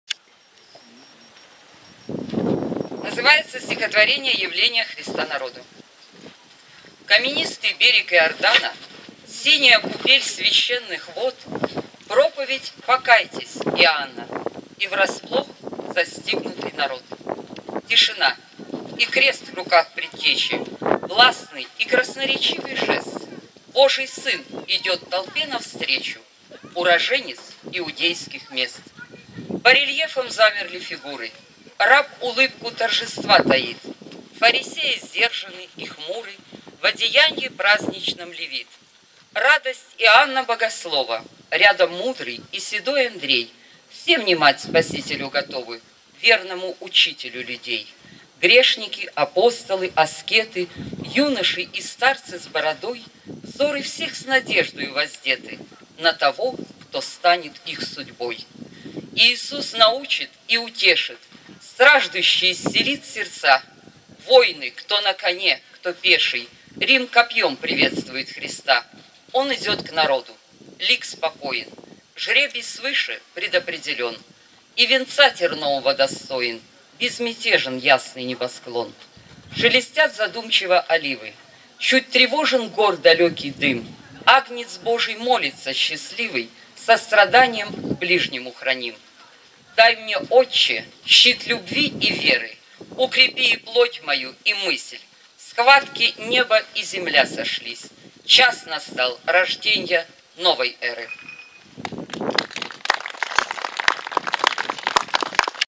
26 CHatyrdag 03062017 Koncert 1
XXIV фестиваль авторской песни «Чатыр-Даг» памяти Людмилы Вертяевой, 2 — 4 июня 2017 года